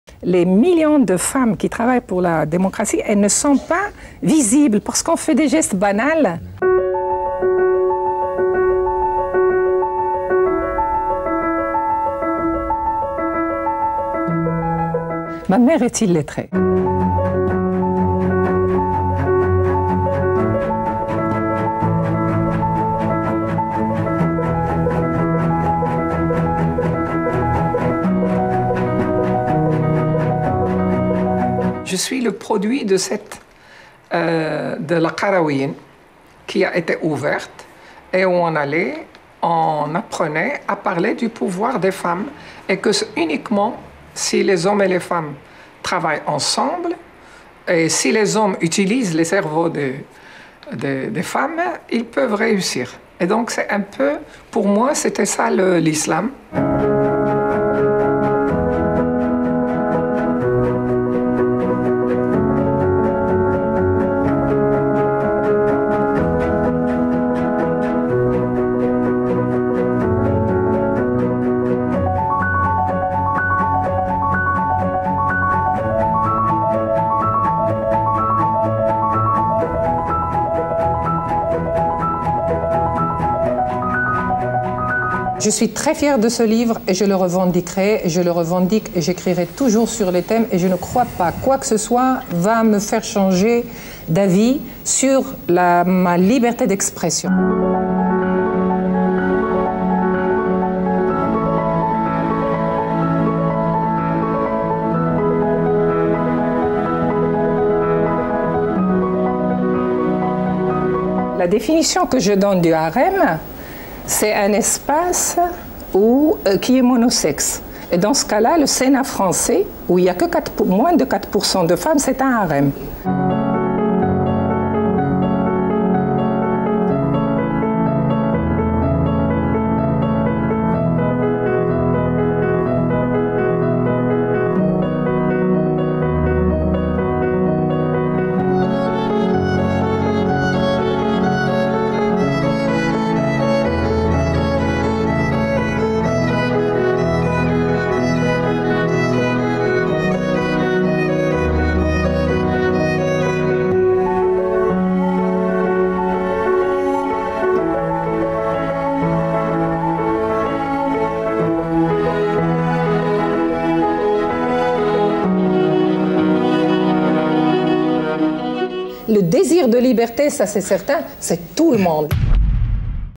Une vie : Fatima Mernissi
Elle a consacré sa vie à défendre la place des femmes musulmanes dans le monde et dans son pays, le Maroc. Portrait de Fatima Mernissi.